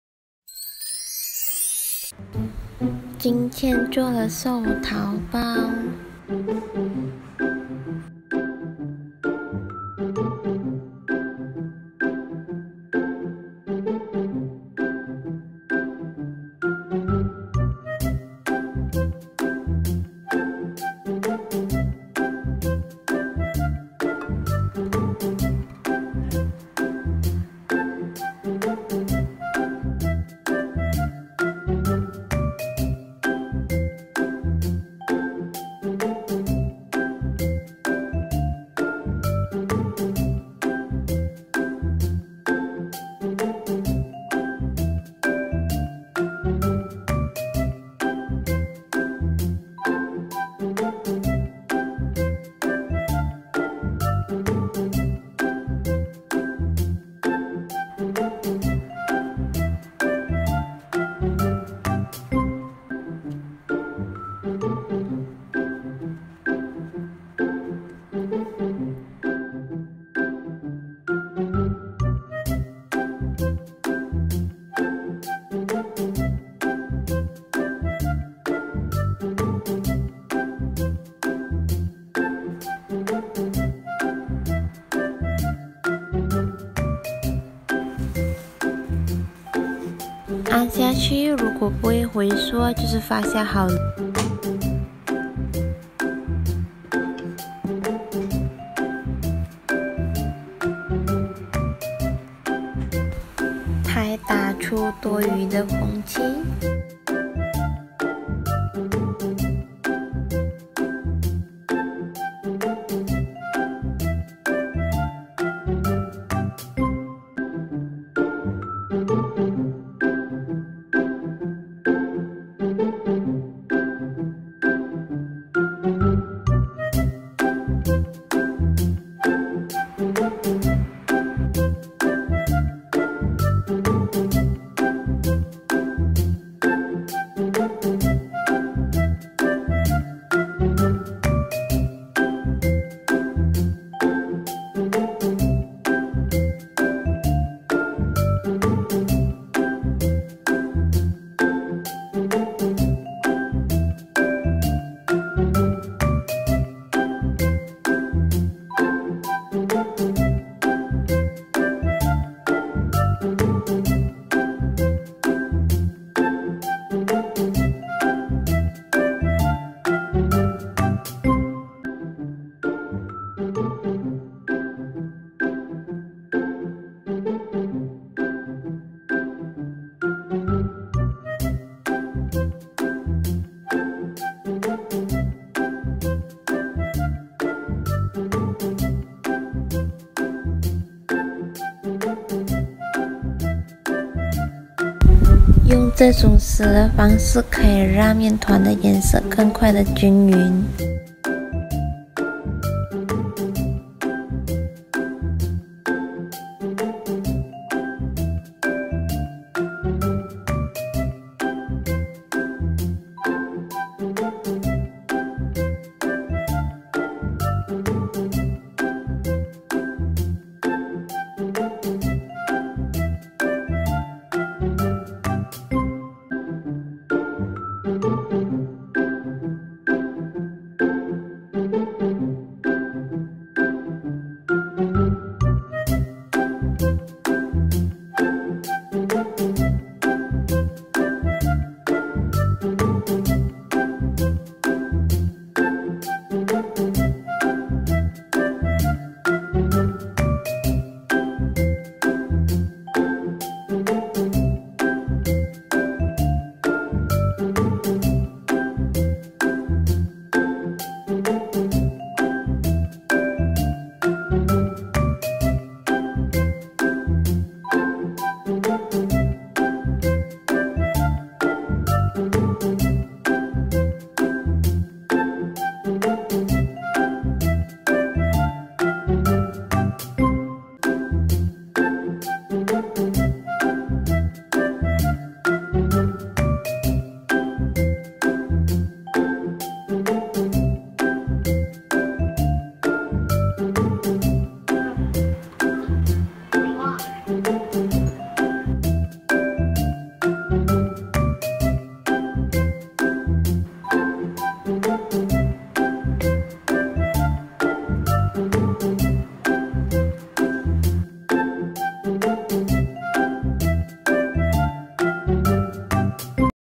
寿桃 | HOMEMADE LONGEVITY PEACH BUNS | EASY RECIPE
I prepared this peach buns for my grandfather 80 years old to celebrate him birthday.

Yield: 14 peach bun

Ingredients:
Salt 3G
Water 250g ( 20g for yeast mixture)
Red bean paste 420g
Matcha powder
Red food colouring

If you have done, you can write a comment share with me. I'm happy that my video can make you to done.
Share with me, if you also DONE it.

MINI PUMPKIN KUIH WITH RED BEAN PASTE  南瓜豆沙糕